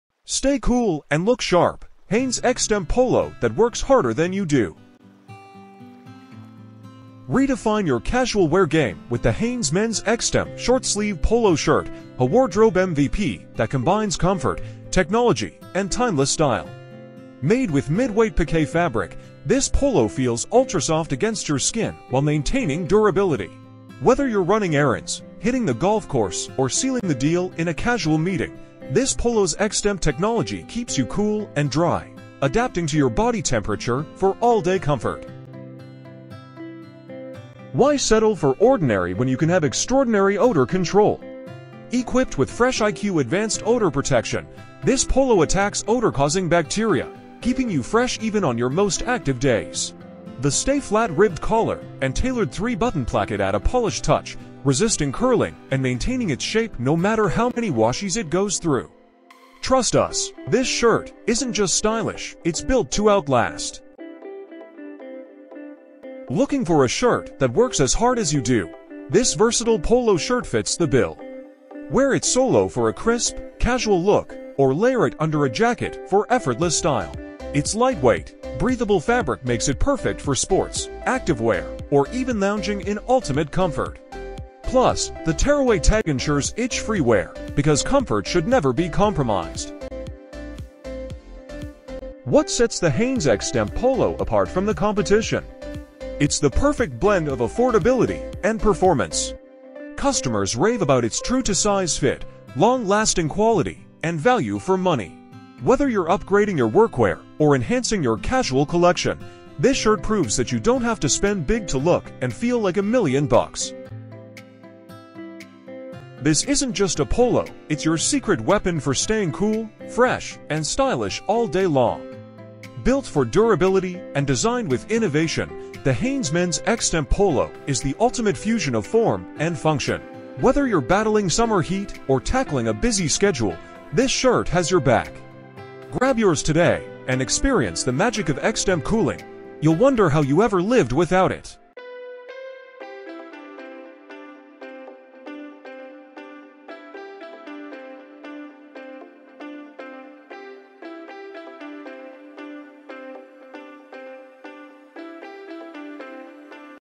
Stay Cool & Look Sharp – Hanes X-Temp Polo That Works Harder Than You Do
Redefine your casual wear game with the Hanes Men’s X-Temp Short Sleeve Polo Shirt, a wardrobe MVP that combines comfort, technology, and timeless style. Made with midweight pique fabric, this polo feels ultra-soft against your skin while maintaining durability. Whether you're running errands, hitting the golf course, or sealing the deal in a casual meeting, this polo’s X-Temp technology keeps you cool and dry, adapting to your body temperature for all-day comfort.

Why settle for ordinary when you can have extraordinary odor control? Equipped with FreshIQ advanced odor protection, this polo attacks odor-causing bacteria, keeping you fresh even on your most active days. The stay-flat ribbed collar and tailored 3-button placket add a polished touch, resisting curling and maintaining its shape no matter how many washes it goes through. Trust us—this shirt isn’t just stylish, it’s built to outlast.

Looking for a shirt that works as hard as you do? This versatile polo shirt fits the bill. Wear it solo for a crisp, casual look, or layer it under a jacket for effortless style. Its lightweight, breathable fabric makes it perfect for sports, active wear, or even lounging in ultimate comfort. Plus, the tearaway tag ensures itch-free wear, because comfort should never be compromised.

What sets the Hanes X-Temp Polo apart from the competition? It's the perfect blend of affordability and performance. Customers rave about its true-to-size fit, long-lasting quality, and value for money. Whether you’re upgrading your workwear or enhancing your casual collection, this shirt proves that you don’t have to spend big to look and feel like a million bucks.

This isn’t just a polo; it’s your secret weapon for staying cool, fresh, and stylish all day long. Built for durability and designed with innovation, the Hanes Men’s X-Temp Polo is the ultimate fusion of form and function. Whether you’re battling summer heat or tackling a busy schedule, this shirt has your back. Grab yours today and experience the magic of X-Temp cooling—you’ll wonder how you ever lived without it!